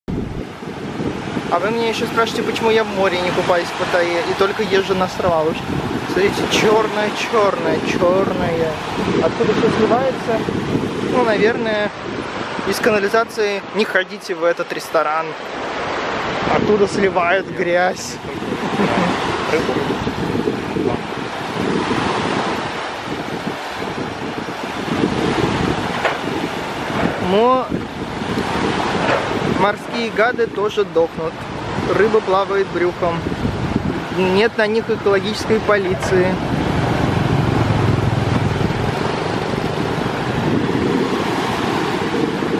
Почему в Паттайе грязное море? Что сливают в море Паттайи?
Что сливают в море Паттайи? Что сливают в море Паттайи?

В открытое море сливают грязь и непонятные выбросы, погибает морская фауна. На видео прекрасно видно рыбу плавающую вверх брюхом. Подробностей со стороны администрации или экологической службы нет.